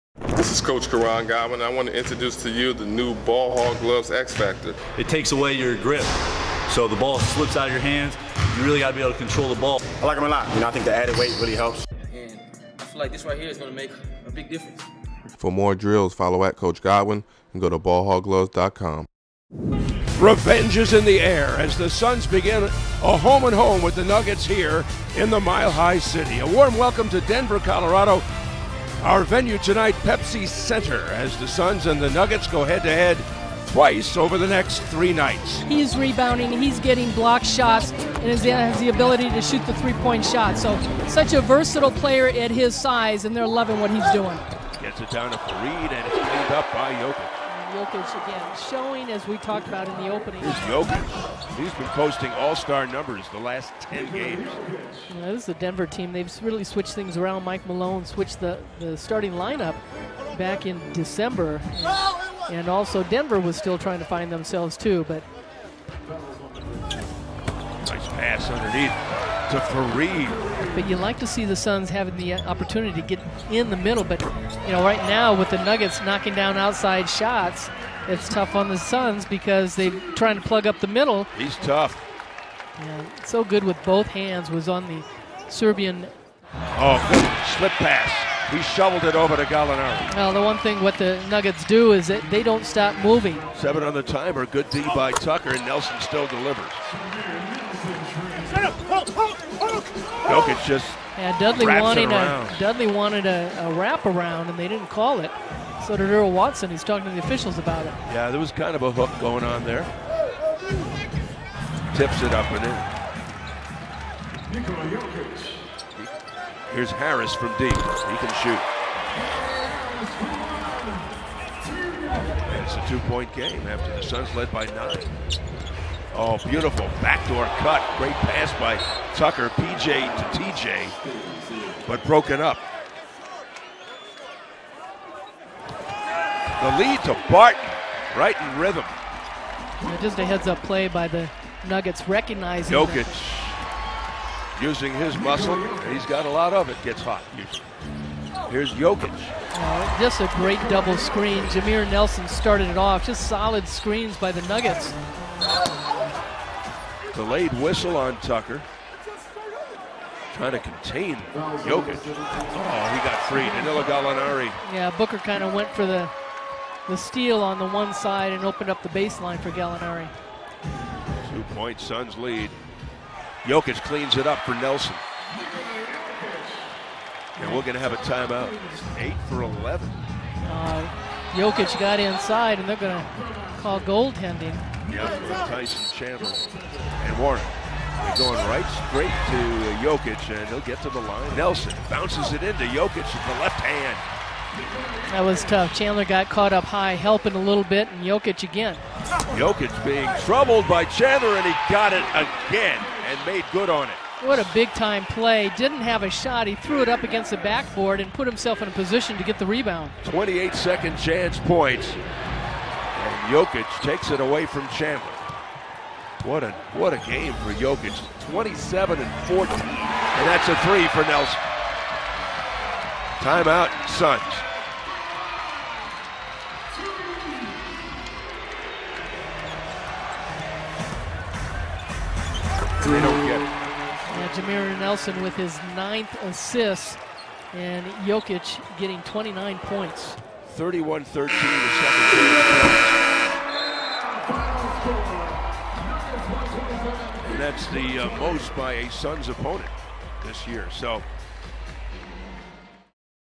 Nikola Jokic Full Highlights vs Suns (2017.01.26) - 29 Pts, 14 Reb, 8 Ast, BEAST!
BE SMARTER! Basketball Brain Workout